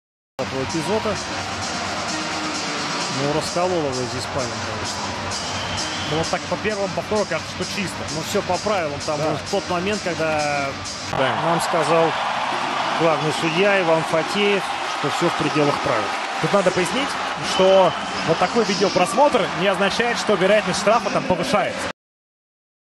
Григорий Панин применил красивый силовой приём против Максима Шабанова 💣💥💯
Григорий Панин применил красивый силовой приём против Максима Шабанова, но, когда судьи отправились смотреть повтор момента, видимо, внутренне уже был готов отправиться в штрафной бокс. Судьи не усмотрели нарушения правил, что Панин оценил по достоинству.